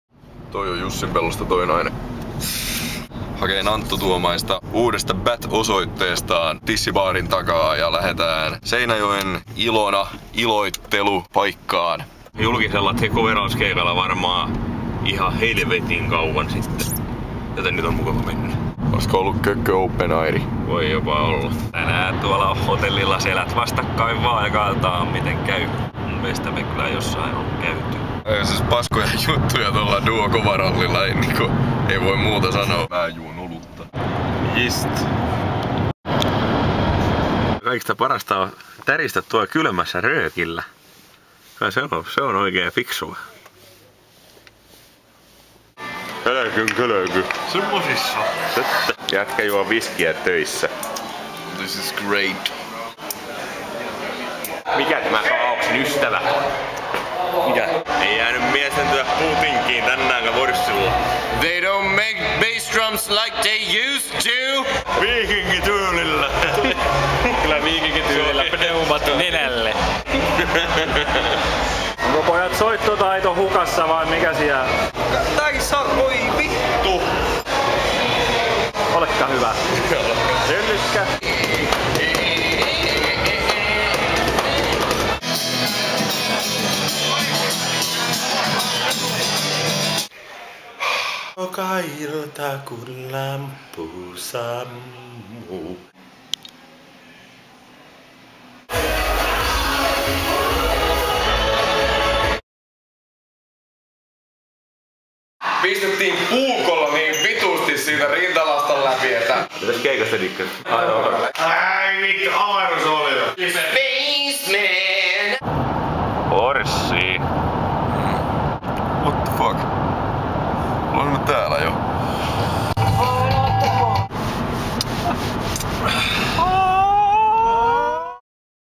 The Coveralls @ Ilona, Seinäjoki, 8.10.2011 (Roadmovie)
Tylsyytensä takia nopeatempoiseksi koostettu roadmovie coverhölmöjen reissaamisesta Seinäjoen Ilonaan.

Keikka oli mainio, mutta siitä ei valitettavasti ollut materiaalia!